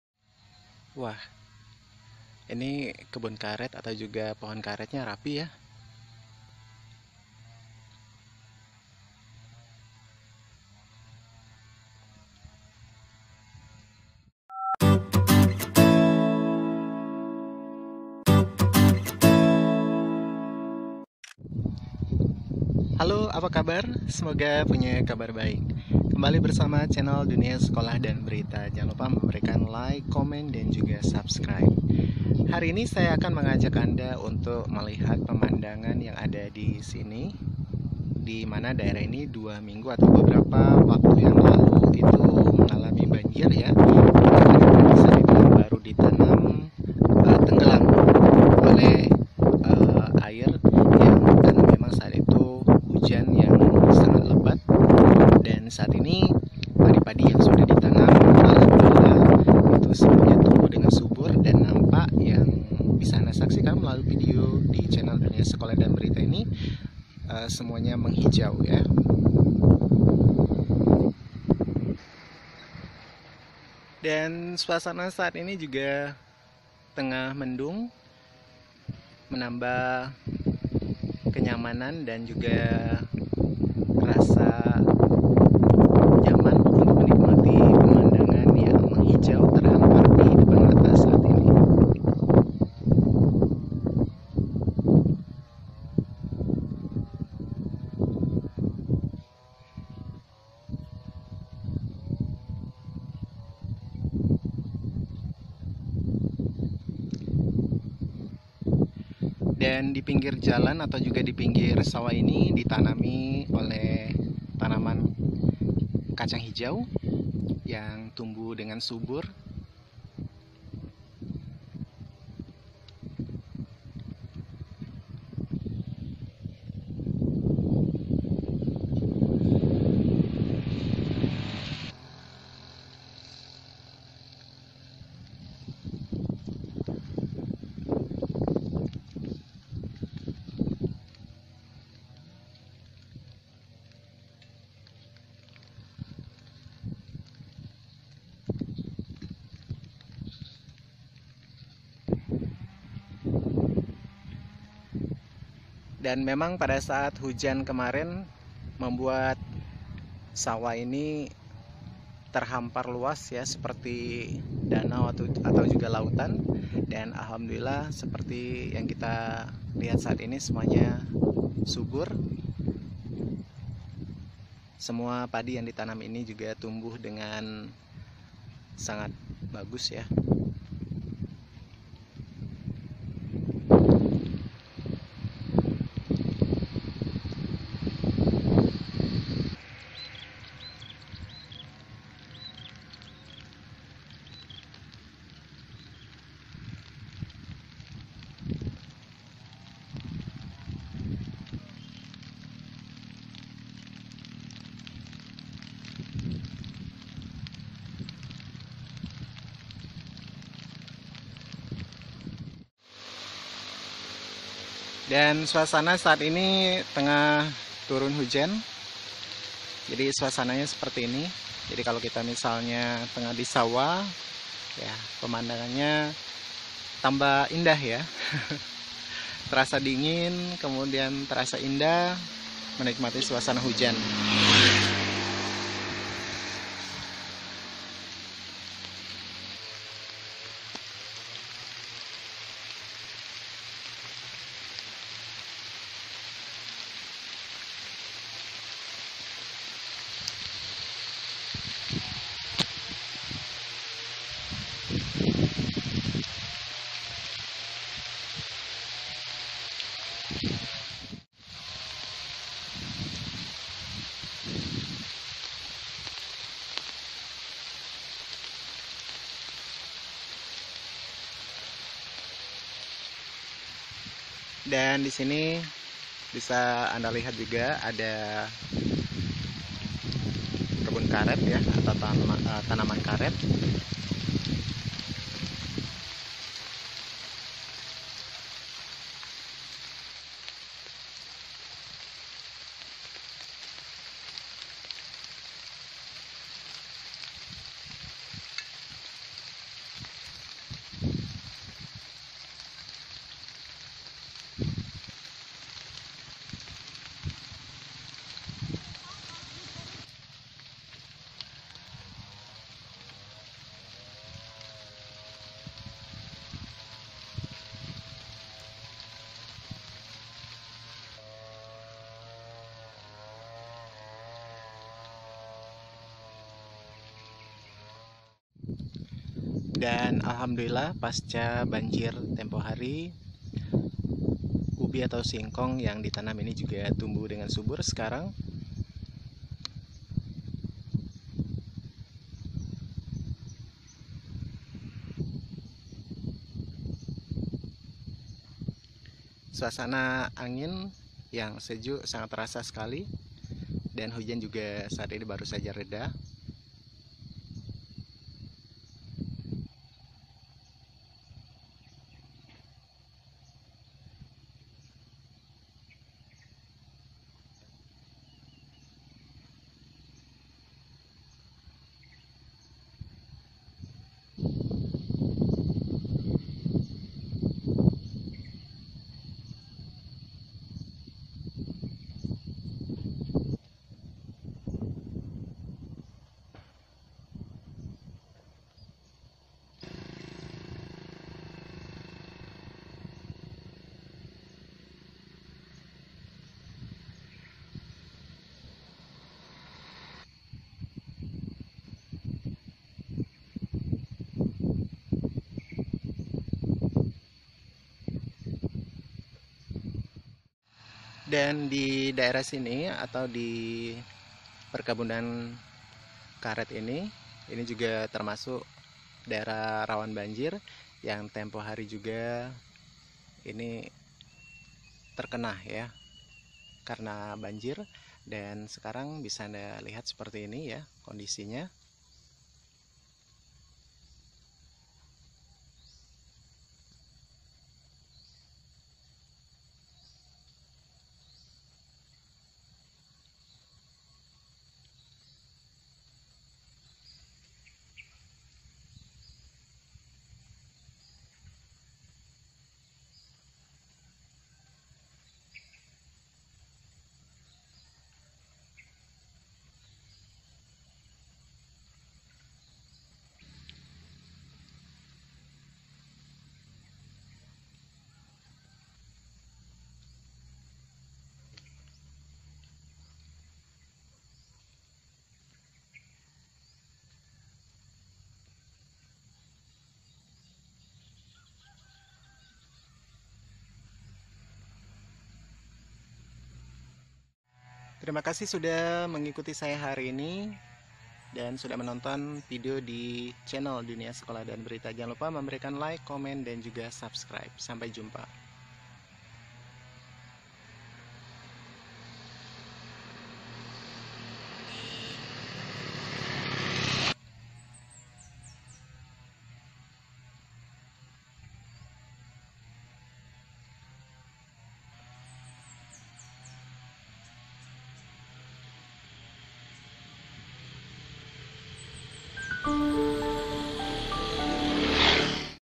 After happened flood now you see a beautiful view | Pasca banjir tanaman subur dan pemandangan indah
Beberapa waktu yang lalu sawah banyak yang terkena banjir, merendam padi yang baru ditanam. Untunglah padi-padi tersebut sudah tumbuh dengan subur. Warnanya yang hijau menjadikan pemandangan semakin indah.

Yuk tonton video ini sampai selesai. Sebuah chanel yang menyajikan info sekolah, vlog, chef dan hiburan untuk anda.

Terima kasih.
Salam.